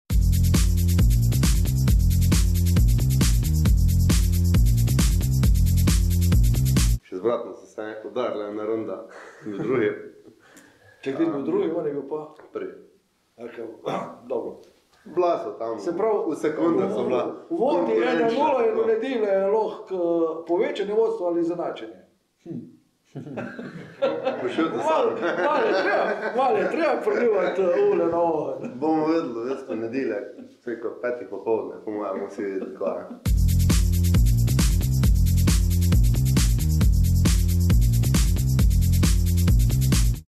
Jan ali Nejc? I Intervju z dirkaško družino Mrak
Intervju z dirkaško družino Mrak je nastal pred 14. Rallyem Idrija.
Andrej, Jan in Nejc so znana dirkaška družina iz Idrije, ki pa so na 14. Rallyu Idrija prvič nastopili hkrati. Dve generacije, trije dirkači in številne zanimive anekdote, spomini in vpogledi v zakulisje najbolj priljubljenega dirkaškega športa pri nas.